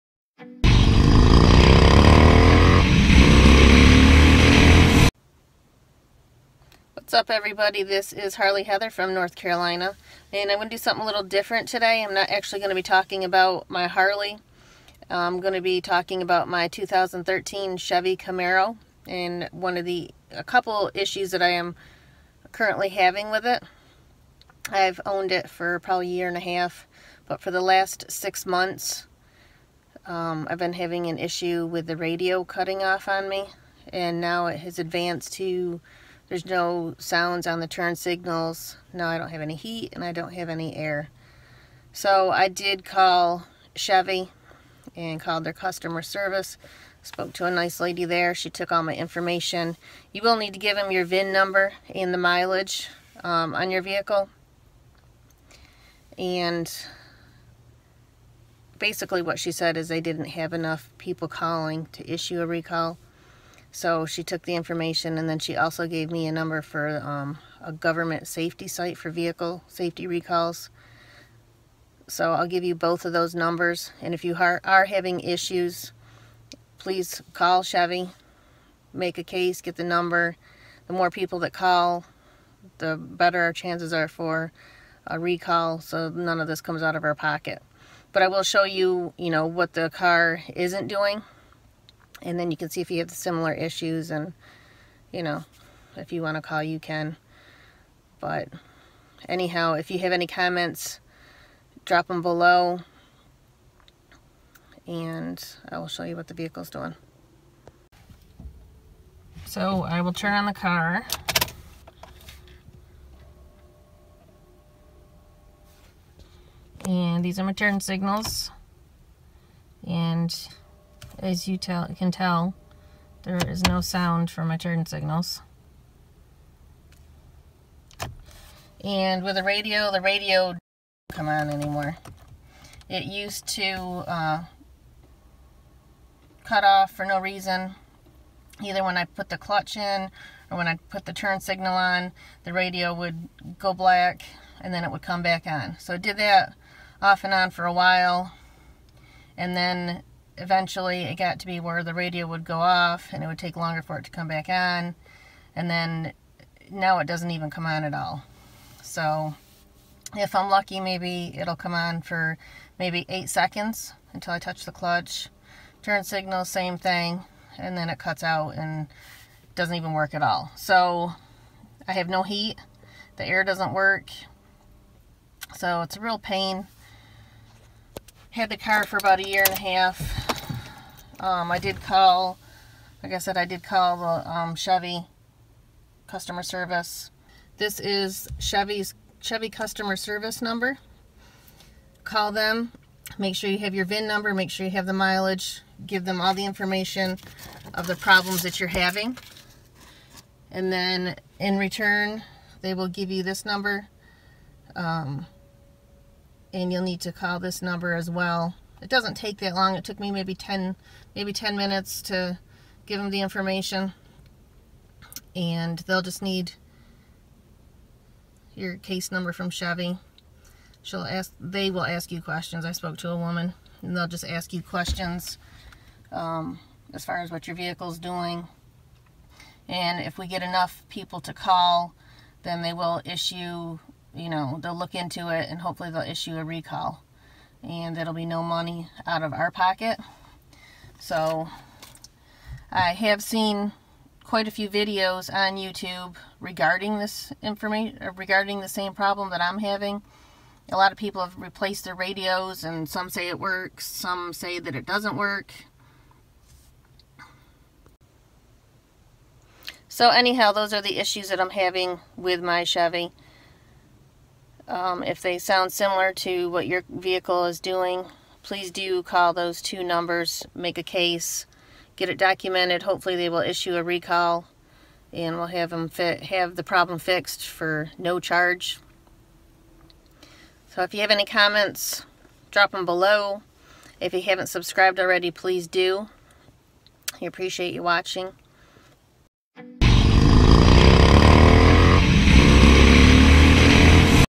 Chevy GMC Camaro Malfunctioning Radio faceplate control panel-known GM Issue
I have a 2013 Chevy Camaro.  I have had radio issues for about the last 6 months.  First started out as cutting off and on all the time. Then it would stop working for longer periods of time. Now it doesn’t come on at all. Using turn signals or pushing in the clutch initially would make the radio stop working.  Now my turn signals have no sound and the heat and air will not work. I called Chevy customer service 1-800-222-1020 and made a claim for my issues and was given a case number. Chevy then gave me a number for NHTSA. This is a government safety recall at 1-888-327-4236. I called that number and gave them my case number . What they do is enter claims and see if there are enough complaints to issue a recall. So, if you are having the same issues please call and report them. The more people that call, the better chances of a recall being issued.

Links:

Bikes:
2018 Harley Davidson Softail Slim
2004 Harley Davidson Sportster Roadster 1200